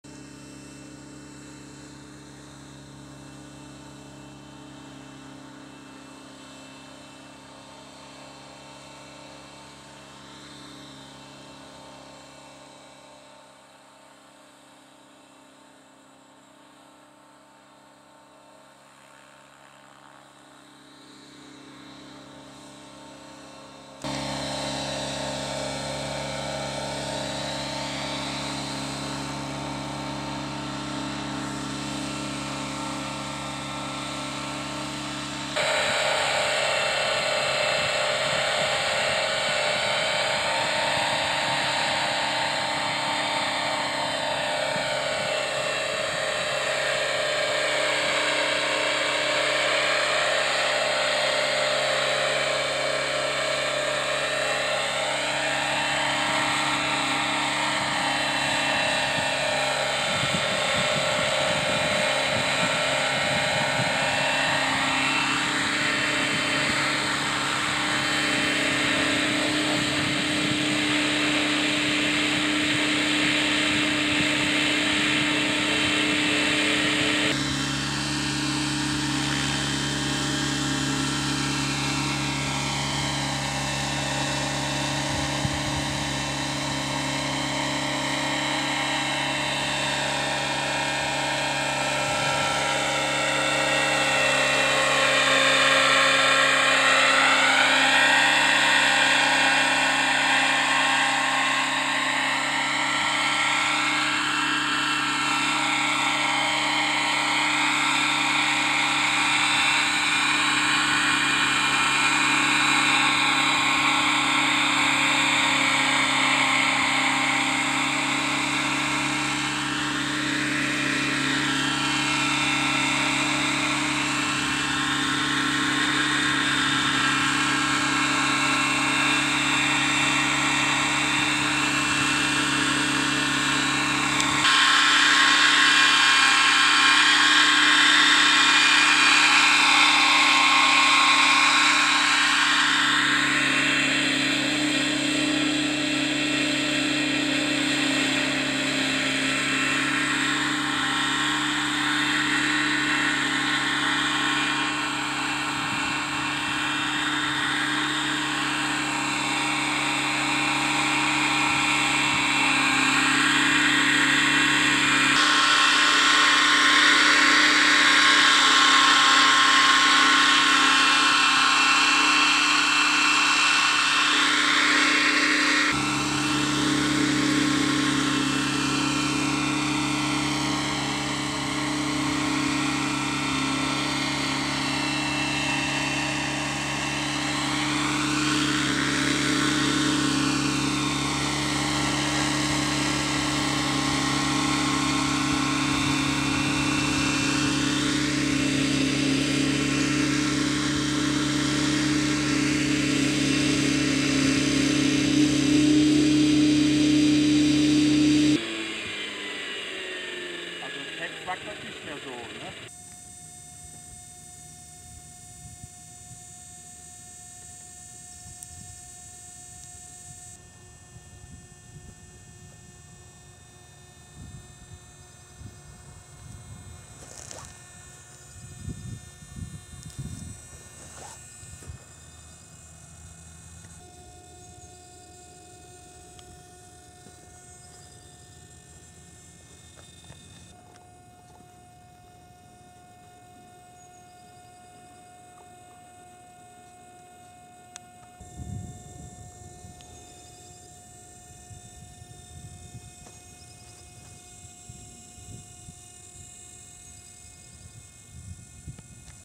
T-Rex 600 EFL Pro auf HUGHES 500E
Bernds T-Rex 600 EFL Pro auf HUGHES 500E
einer mit den ersten Schwebeflügen nach der Winterpause
demnächst gehts wieder ab.
Viel Erfolg Bernd !!!